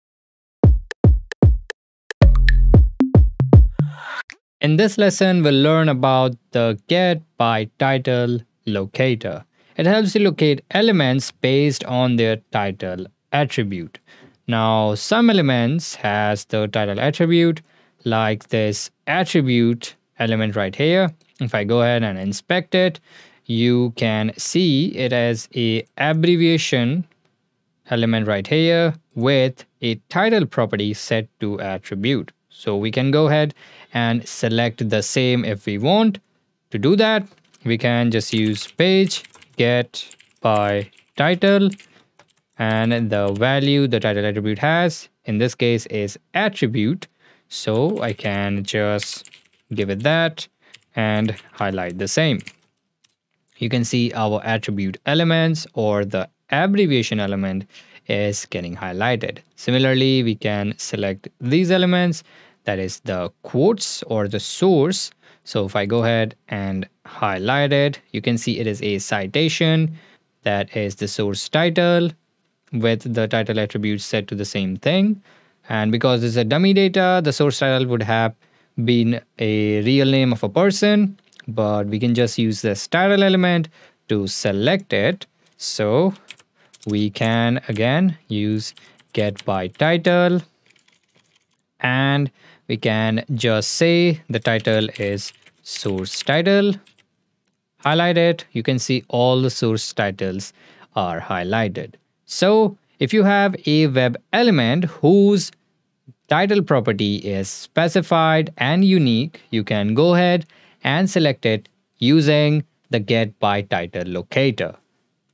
Playwright Python - Title Locator
In this video you'll learn how to locate web elements based on their title attribute.

Locate an element with a matching title attribute using page.get_by_title().
The title attribute is commonly used in web elements to provide additional information and create tooltips.

It can be applied to various HTML elements such as links, buttons, inputs, and images. When a user hovers over or interacts with an element, the title attribute displays a brief description or contextual information.

It helps improve user experience by providing extra details without cluttering the interface. However, it's important to use the title attribute judiciously, keeping tooltips concise and accessible to all users.

Use this locator when your element has the title attribute.

Playwright is a modern, fast and reliable browser automation tool from Microsoft that enables testing and automation across all modern browsers including chromium, firefox and webkit.